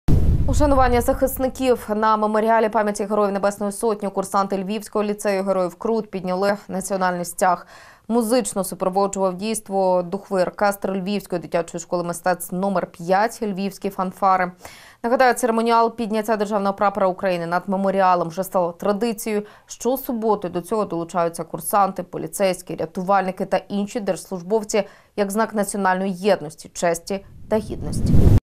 Ушанування захисників / Випуск новин 16.11.2020
На Меморіалі пам’яті Героїв Небесної Сотні курсанти Львівського ліцею Героїв Крут підняли національний стяг.